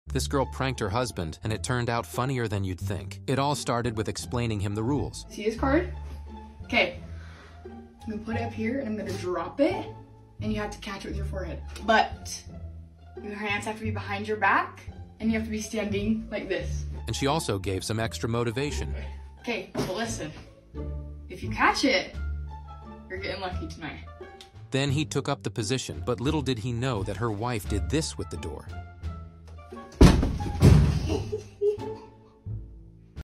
WAIT until the END🤣
This girl pranked her husband, and it turned out funnier than you’d think. After he arrived she explained him the rules: And she also gave some extra motivation: So he took up the position, but little did he know, that her wife did THIS with the door:

📸: moandmiss on TT with permission